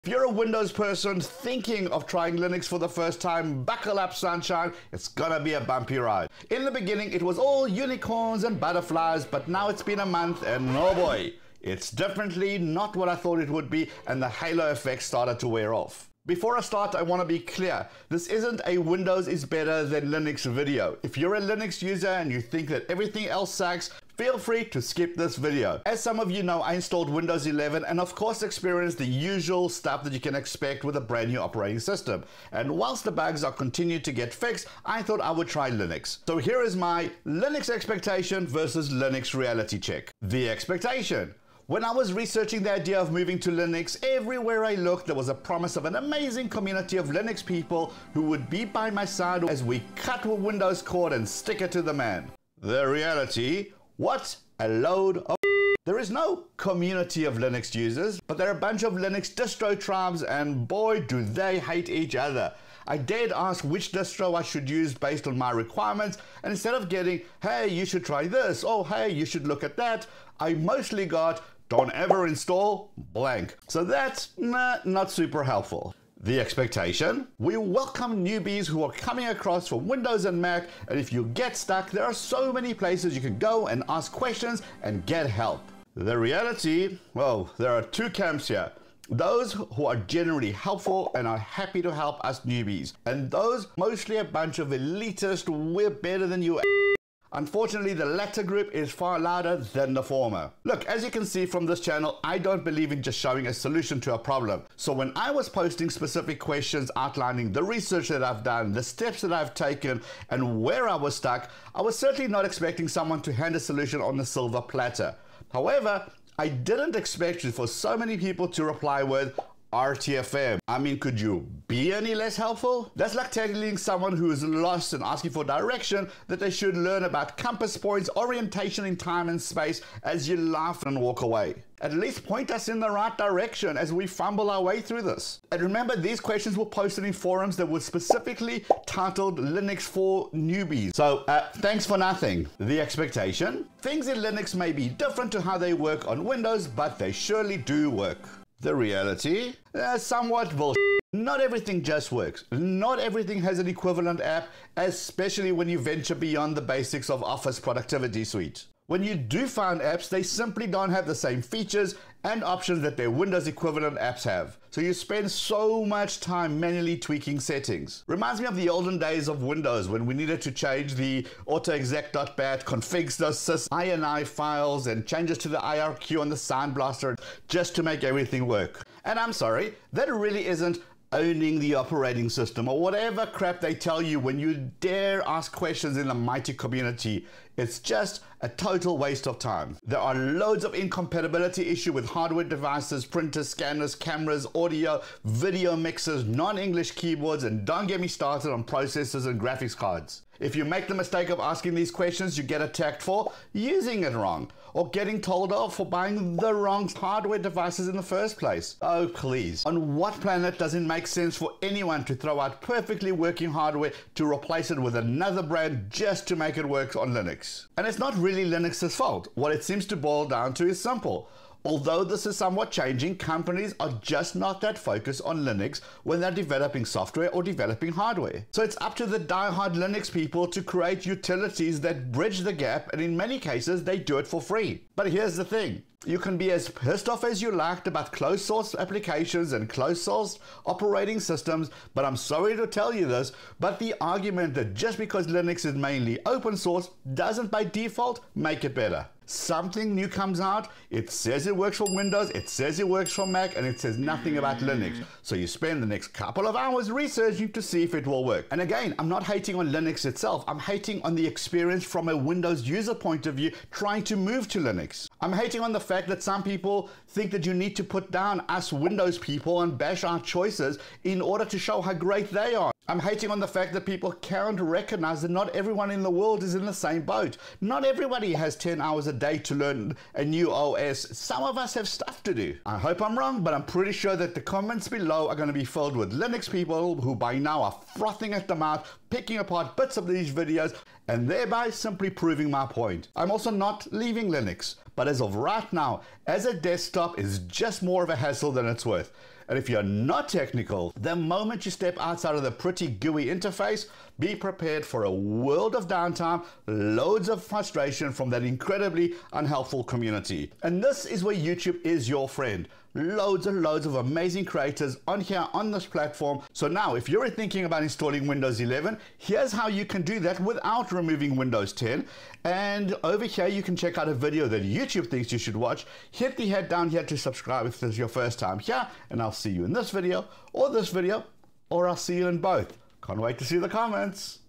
Don't switch to Linux - it sucks for most people!
I tried to switch over to Linux and the reality vs expectation is just on another level...

0:00 How hard is it to switch to Linux from Windows?
0:40 The Linux Community
1:17 Dumb noob question
2:25 Everything on Linux works
3:38 Linux Fault
4:50 YouTube is Linux friendly